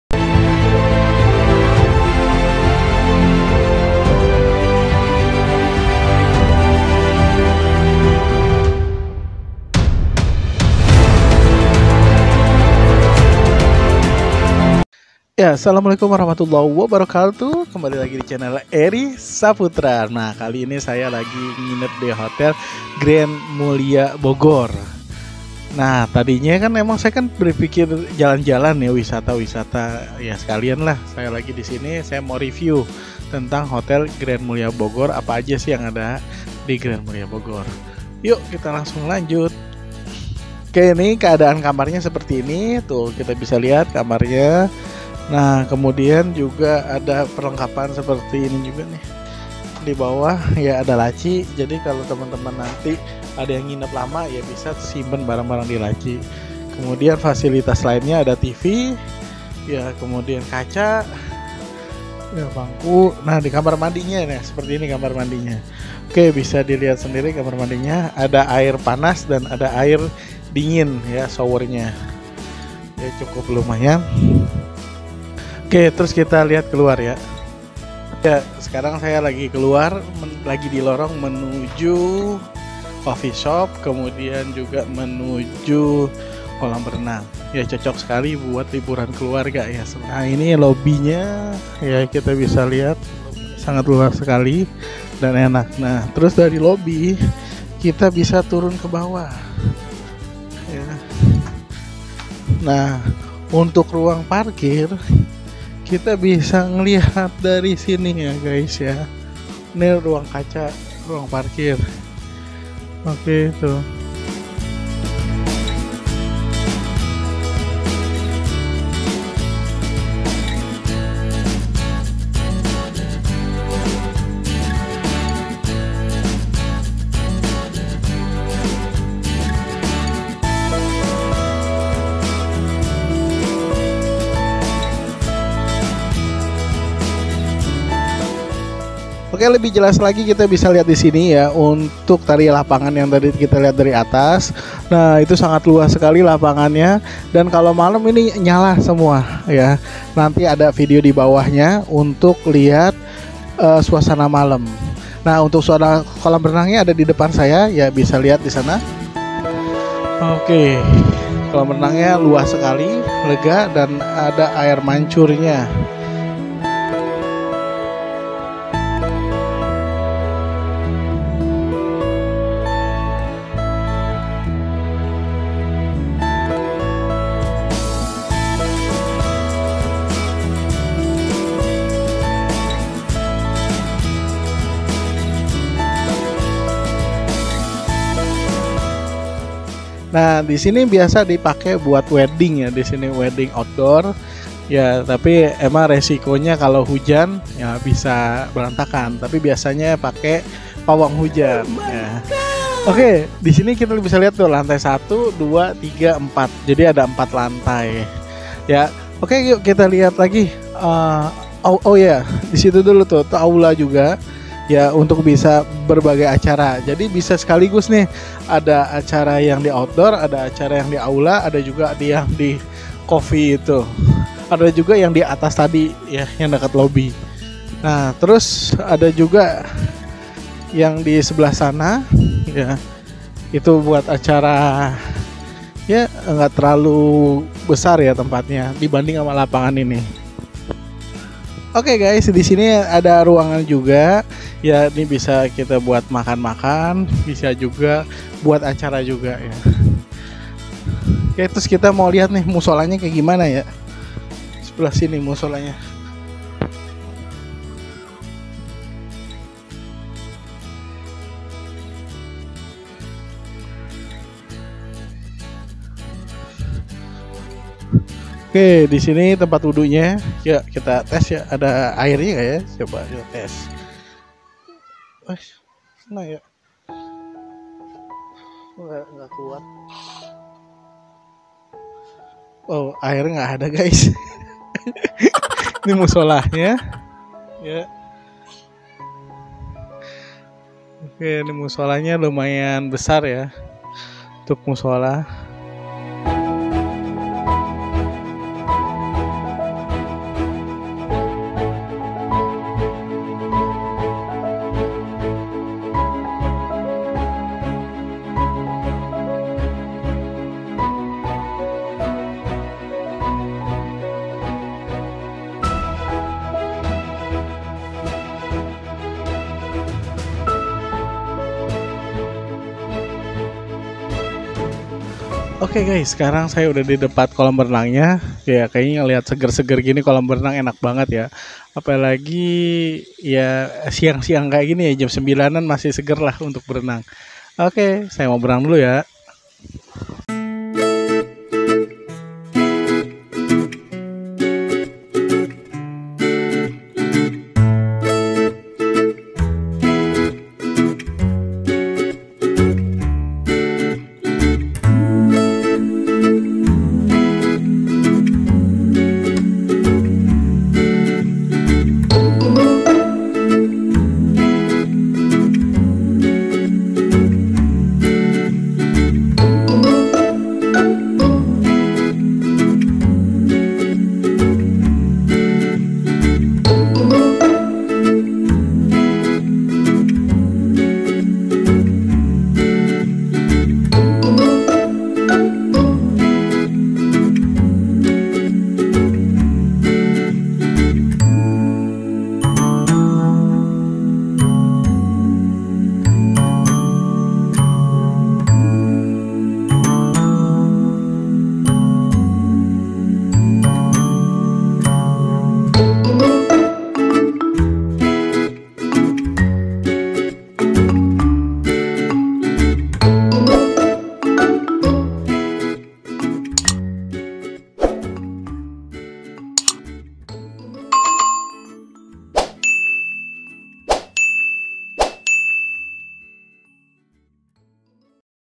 Review Hotel Grand Mulya Bogor | hotel murah kualitas istimewa
bagi kamu yang hobby nya jalan-jalan, silahkan mampir nih,,, ke hotel Grand Mulya Bogor, hotelnya nyaman suasana bogor yang adem, untuk booking bisa via online ya

untuk kontak saya bisa di terima kasih